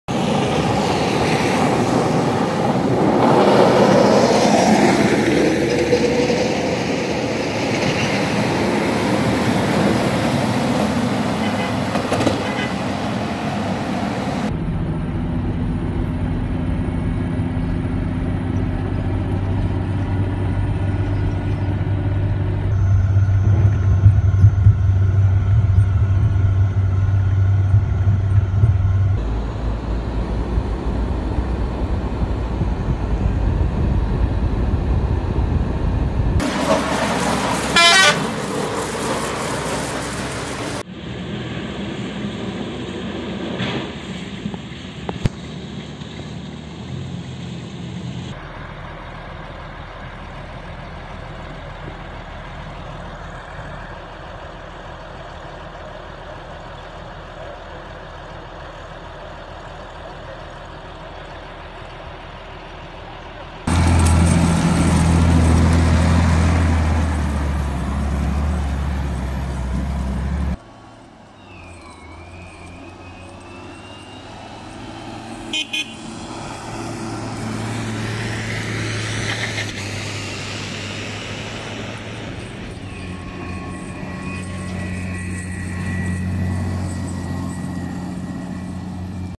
Büyükşehir Yoğun Kar Yağışına Meydan Okuyor
Yoğun kar yağışı, tüm yurtta olduğu gibi Ordu’da da etkisini gösteriyor. Ordu Büyükşehir Belediyesine bağlı ekipler yol açma çalışmalarına aralıksız devam ediyor.